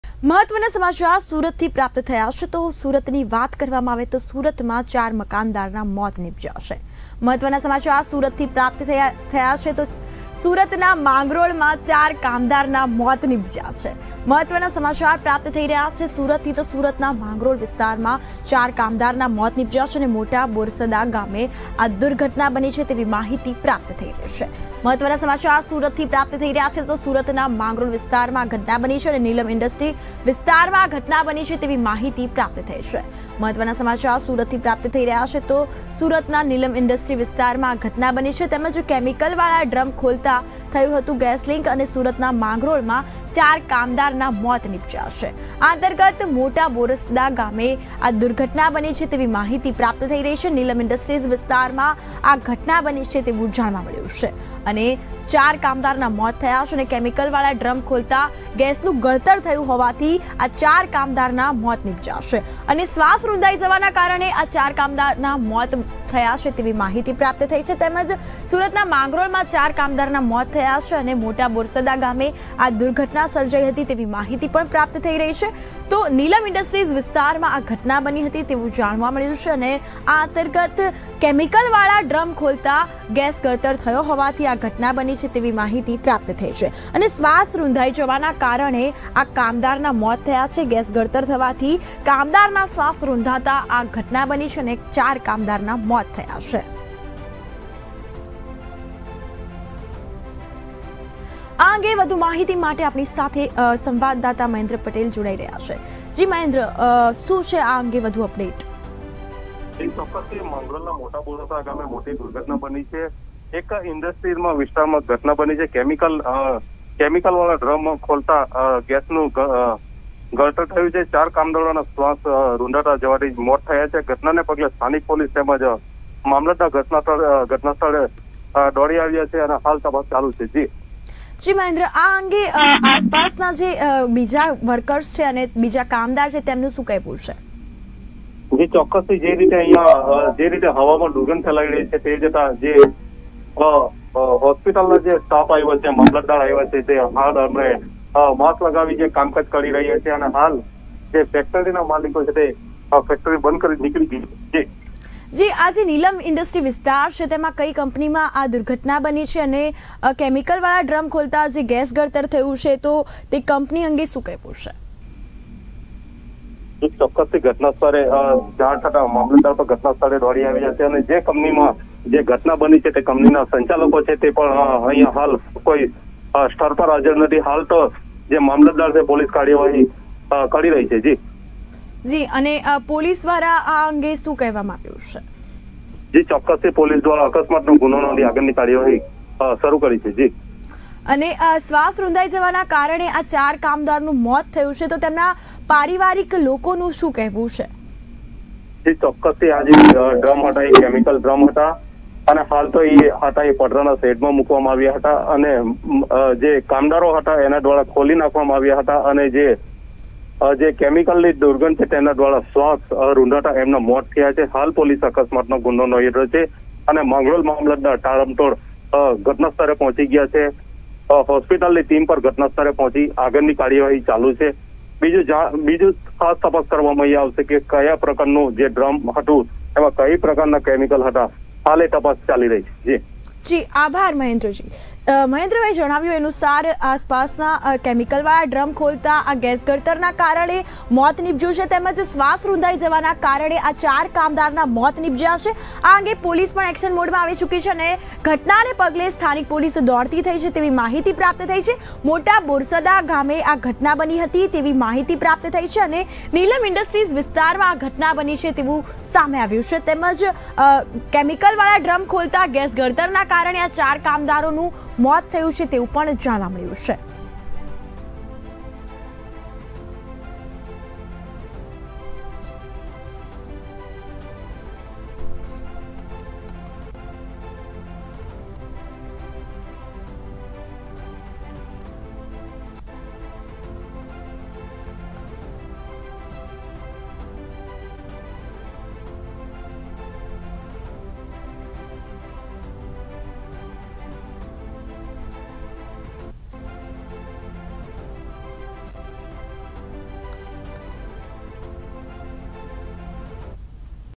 સુરતના માંગરોળમા 4 કામદારના મોત
SURAT : સુરતના માંગરોળમા 4 કામદારના મોત
મોટા બોરસરા ગામે બની દુર્ઘટના
નીલમ ઇન્ડસ્ટ્રીઝ વિસ્તારમાં બની ઘટના
કેમિકલવાળા ડ્રમ ખોલતા થયું ગેસ ગળતર
શ્વાસ રૂંધાઇ જવાના કારણે 4 કામદારના મોત
ઘટનાને પગલે સ્થાનિક પોલીસ દોડતી થઇ